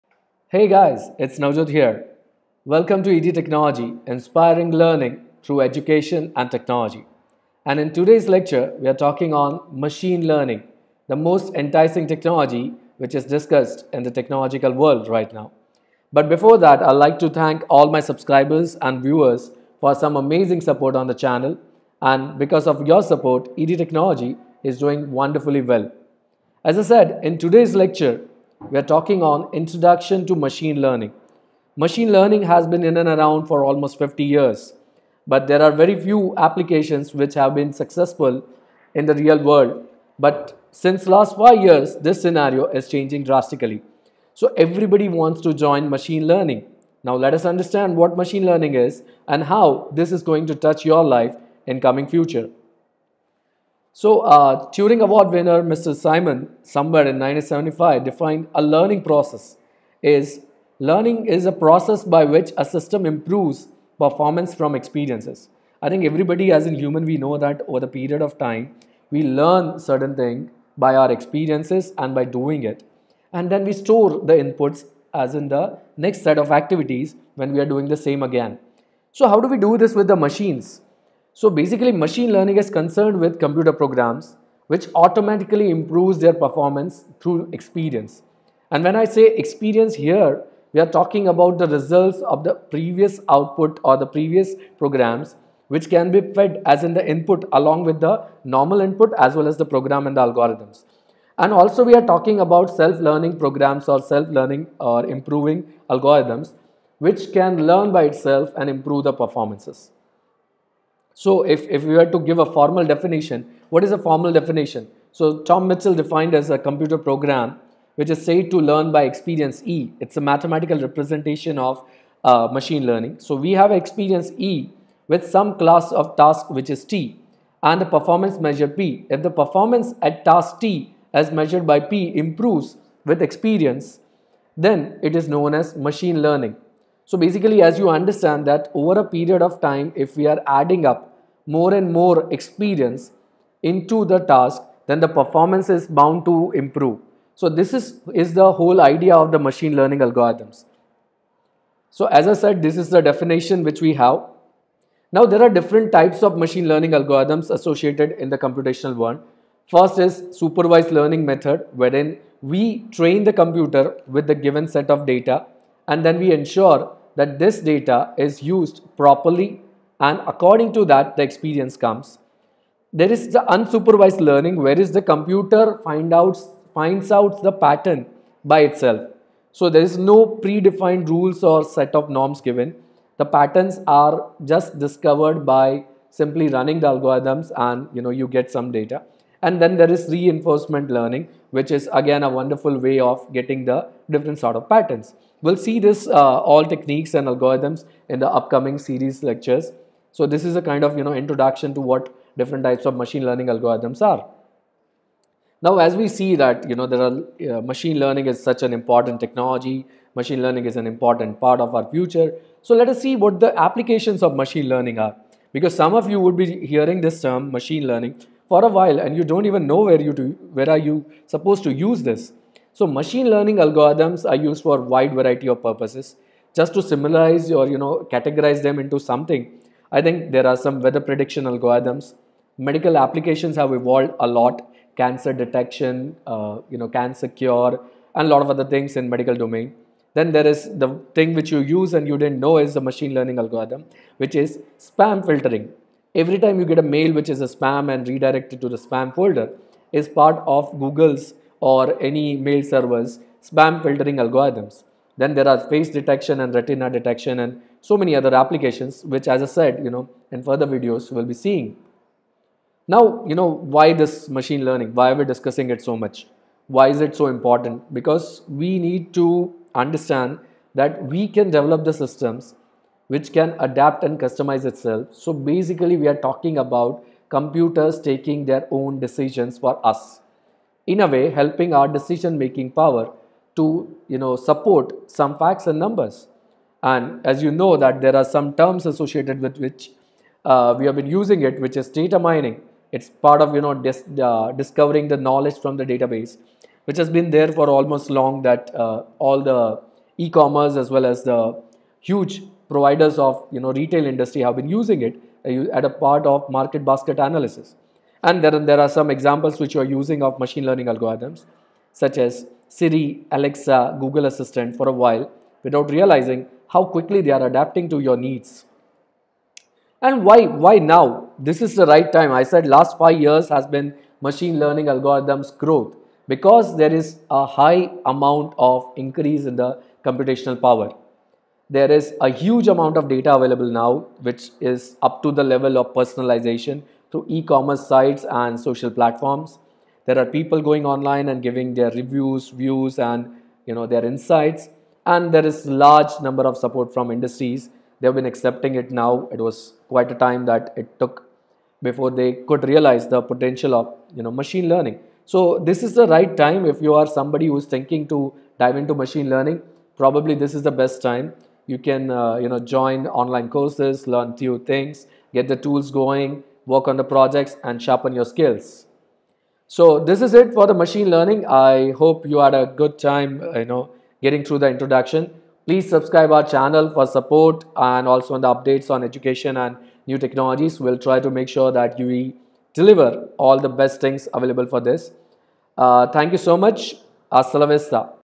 What is Machine Learning? | Introduction to Machine Learning | Machine Learning Lectures
What is Machine learning? Why Machine Learning? Types of Machine Learning Algorithms and everything you need to know to start Machine Learning. Supervised learning and unsupervised learning techniques. various ML techniques!